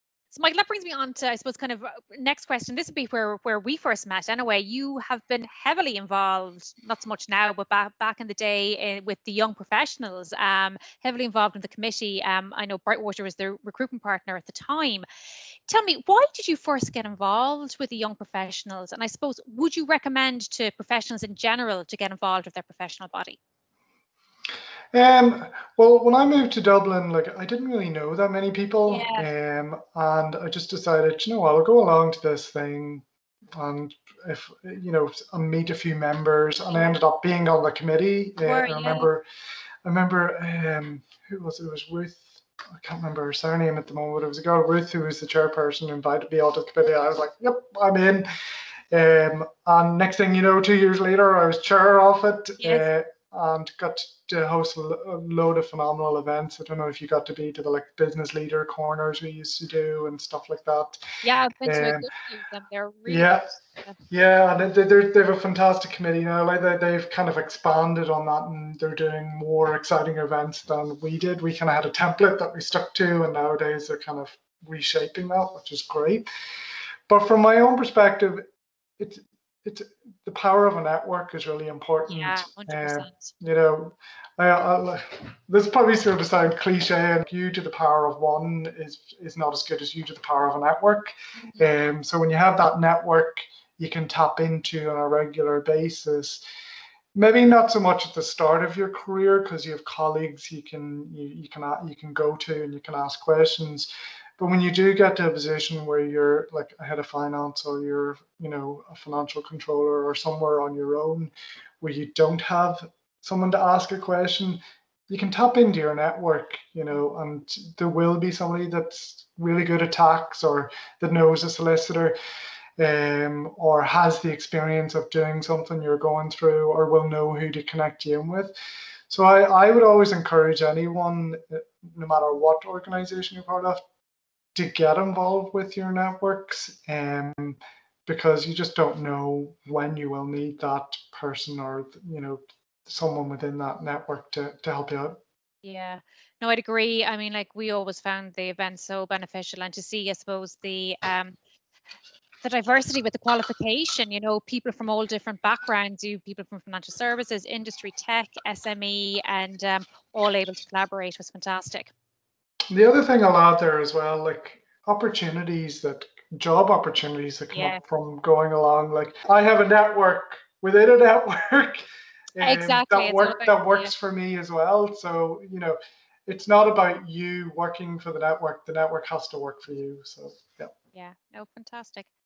Would you recommend professionals to get involved with their professional body?
JOD: That brings me onto my next question and this is where we first met. You have been heavily involved, (not so much now but back in the day) in the Young Professionals, heavily involved with the committee where Brightwater was the recruitment partner at the time. Tell me, why did you first get involved with the Young Professionals and would you recommend in general for professionals to get involved with their professional body?
MJW: Well, when I first moved to Dublin, I didn’t really know anybody so I thought I’d go along to this thing and meet a few members. I ended up being on the committee. I remember a girl Ruth who invited me onto the committee and I thought “yep, I’m in”. Next thing you know, two years later, I was chair of it and got to host a lot of phenomenal events. I don’t know if you got to be involved with the Business Leaders Corner.
JOD: Yes, we went to a good few of them, they were really good.
MJW: They’ve a fantastic committee, they’ve kind of expanded on that and they’re doing more exciting events. We had a template that we stuck to and nowadays they’re reshaping that which is great. But from my own perspective, the power of a network is really important.
JOD: Yes, 100%
 MJW: You to the power of one is not as good as you to the power of a network so when you have that network, you can tap into on a regular basis. Maybe not at the start of your career because you have colleagues you can go to and ask questions but when you do get to a position where you’re Head of Finance or you’re a Financial Controller or somewhere on your own, where you don’t have someone to ask a question, you can tap into your network and there’ll be someone who’s really good at tax or who knows a solicitor or has the experience of doing something you’re going through or will know who to connect in with. I would always encourage anyone, no matter what organisation you’re part of, to get involved with your networks because you don’t know when you will need that person or someone within that network to help you out.
JOD: I’d agree. We always found the events so beneficial and to see all the diversity with the qualification from all different backgrounds, you had people from financial services, industry, tech, SMEs, all able to collaborate which was fantastic.
MJW: The other thing I’d add there is the opportunities, job opportunities that come up from going  along. I have a network within a network that works for me as well. It’s not about you working for the network, the network has to work for you as well.